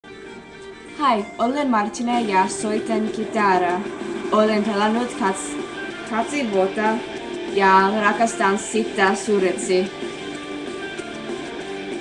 Me speaking Finnish
littlerudegirrrrl's webcam video June 13, 2011 07:59 AM

Hi I'm Martine and i play guitar. I've been playing for 2 years and i love it very much.